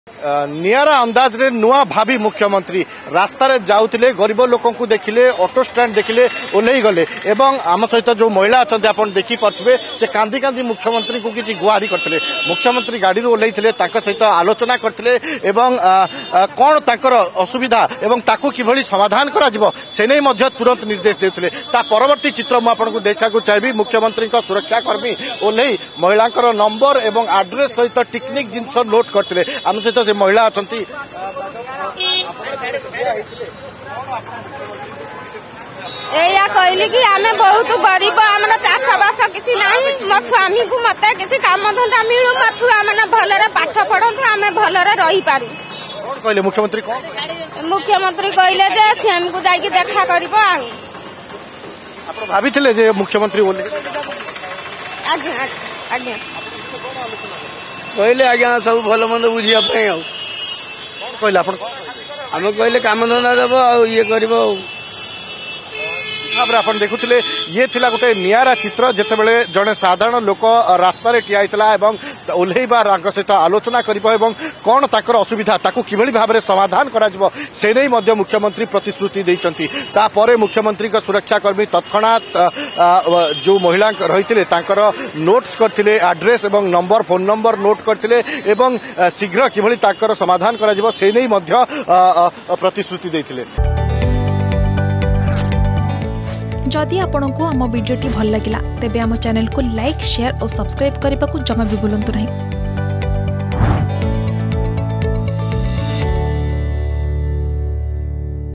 ଅଟୋ ଷ୍ଟାଣ୍ଟରେ ଓହ୍ଲାଇ ଅଟୋ ଚାଳକ ଓ ସାଧାରଣ ଲୋକଙ୍କ ସହ ଆଲୋଚନା କଲେ ନୂଆ ମୁଖ୍ୟମନ୍ତ୍ରୀ || Mohan Charan Majhi
ଅଟୋ ଷ୍ଟାଣ୍ଟରେ ଓହ୍ଲାଇ ଅଟୋ ଚାଳକ ଓ ସାଧାରଣ ଲୋକଙ୍କ ସହ ଆଲୋଚନା କଲେ ନୂଆ ମୁଖ୍ୟମନ୍ତ୍ରୀ ମୋହନ ମାଝୀ ।